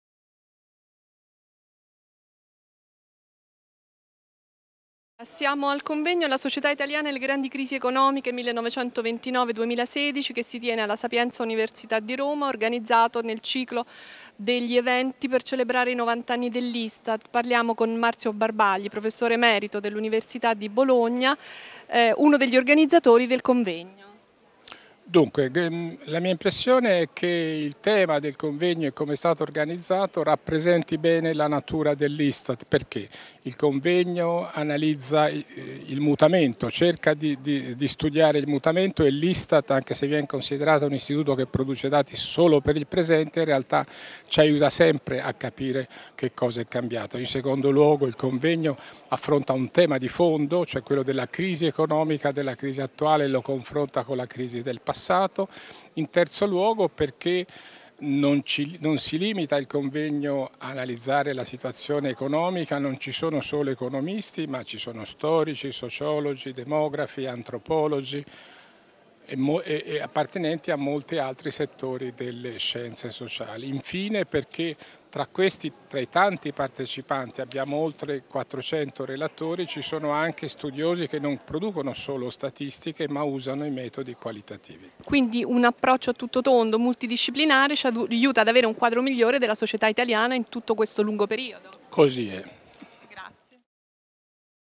Marzio Barbagli | Professore emerito dell’Università di Bologna
Video intervista realizzata in occasione del convegno | La Società italiana e le grandi crisi economiche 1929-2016